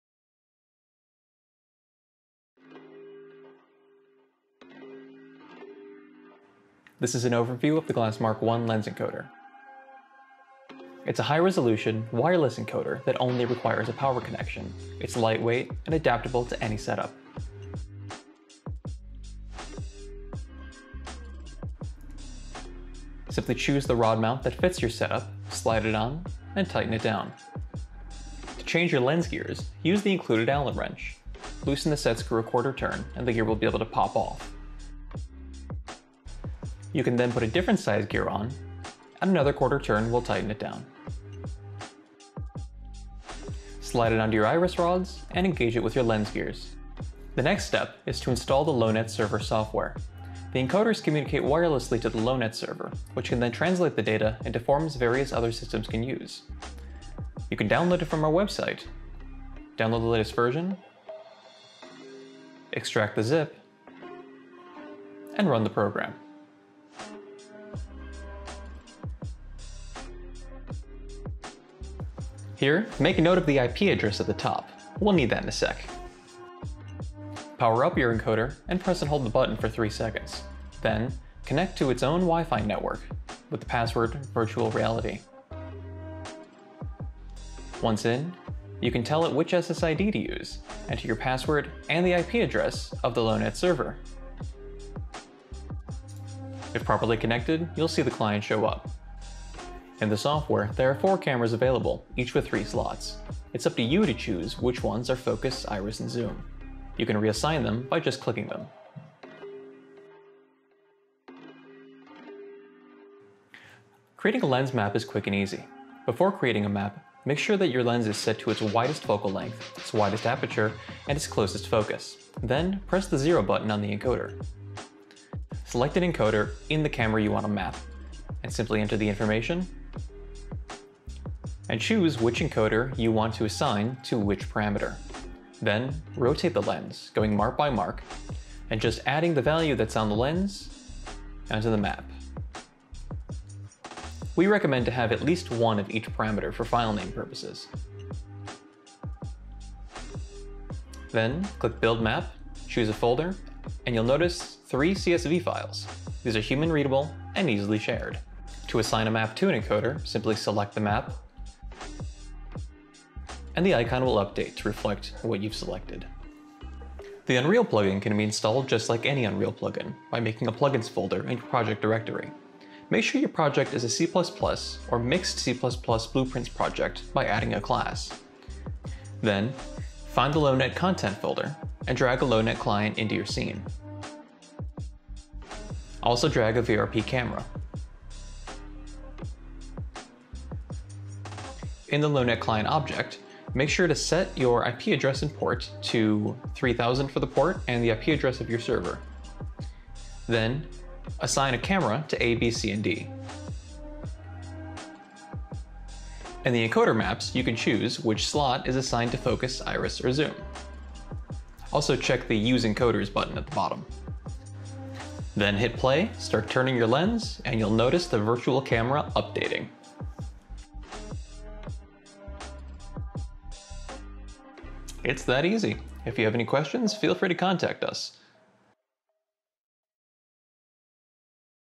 Glassmark I Lens Encoder | Setup and Use
The Glassmark I lens encoder is an advanced solution for virtual production!